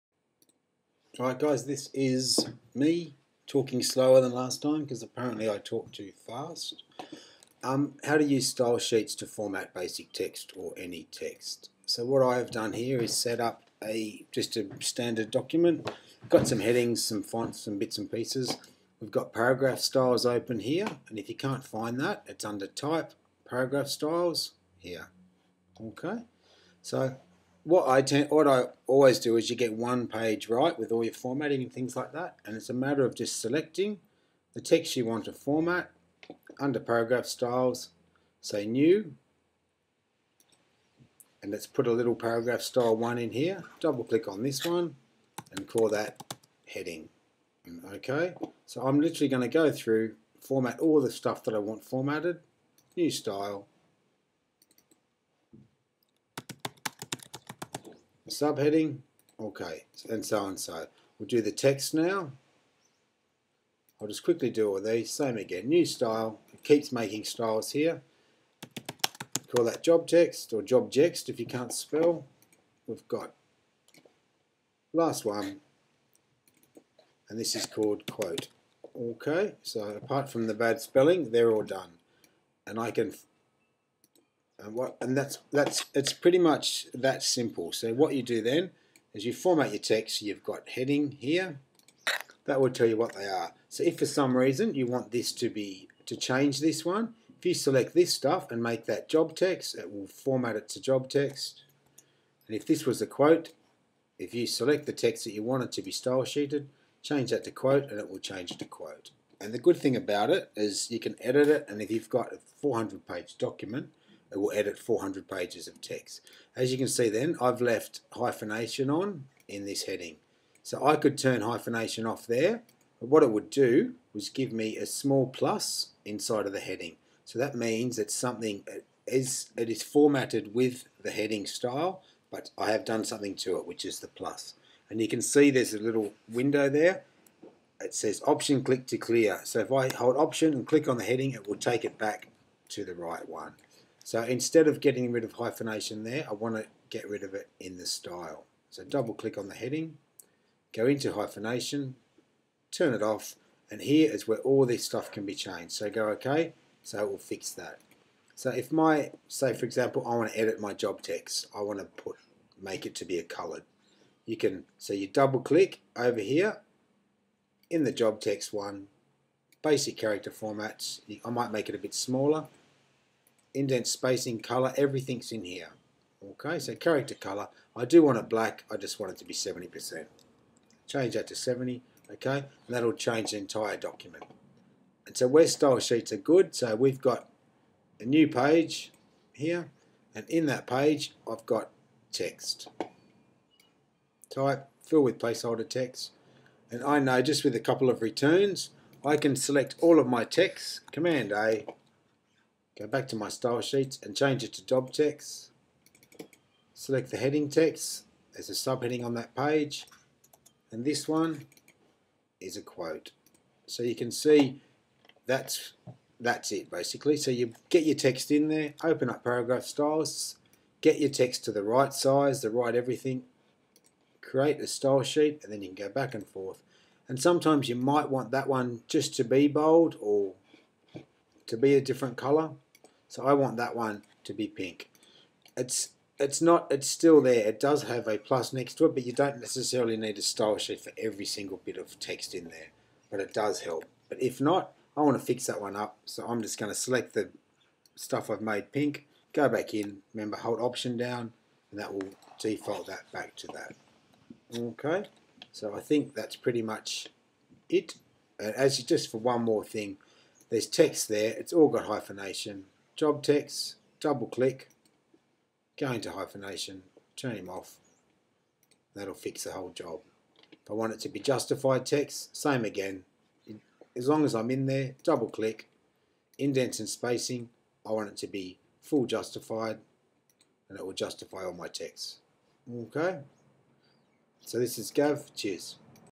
Indesign Using Paragraph Styles
Basic video of setting up Paragraph styles in Adobe Indesign by Gaven Hempstead